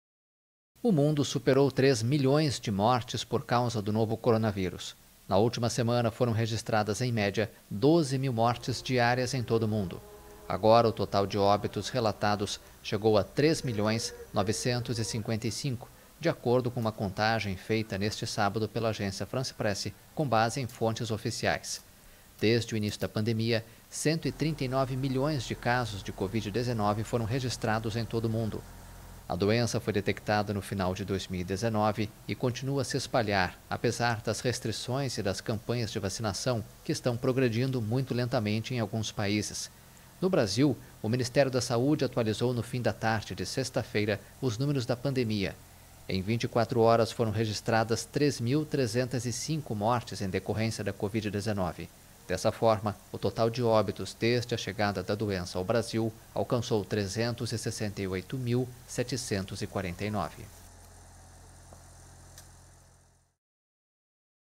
Mundo supera 3 milhões de mortes por coronavírus | AFP
A Covid-19 já matou mais de três milhões de pessoas em todo o mundo. Só no Brasil, o total de óbitos é de 368.749, conforme dados divulgados na última sexta-feira.